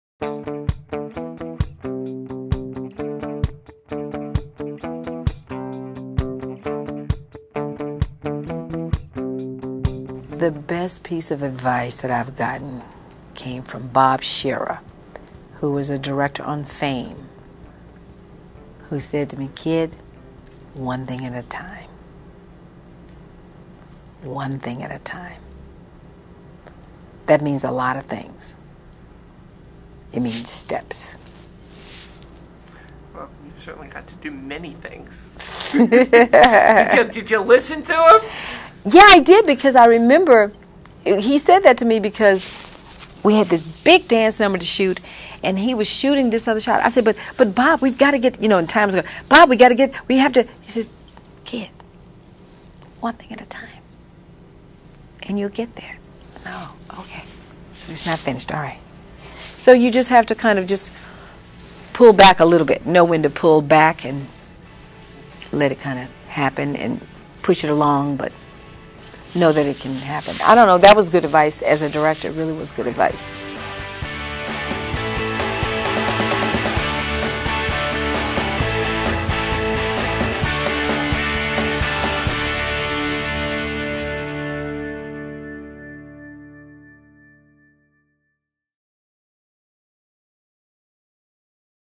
Debbie Allen Tells "The Best Advice I Ever Got"
Debbie Allen (Fame) relates the best advice anyone ever gave her.